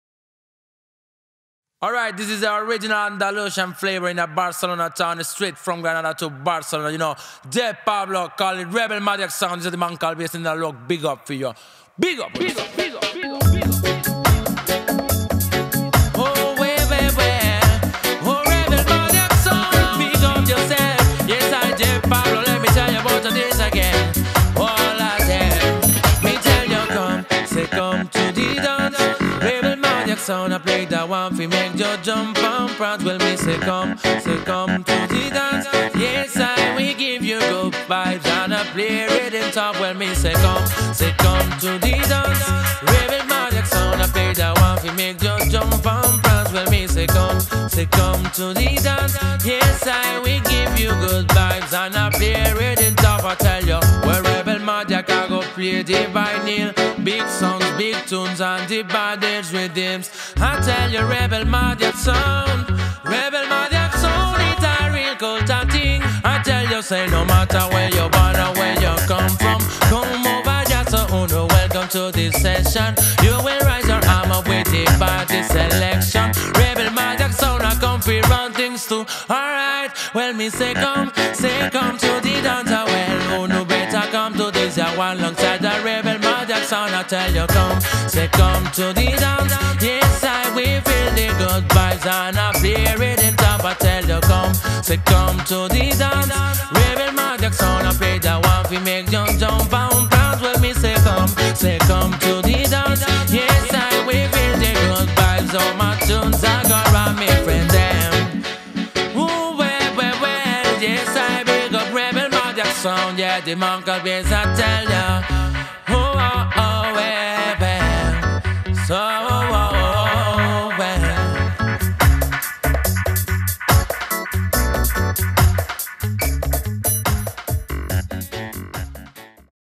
Rebelmadiaq Dubplate Video Collection. Vol 4. ALEX BASS
Dubplate.
Rebelmadiaq meets Alex Bass (Barcelona 2016)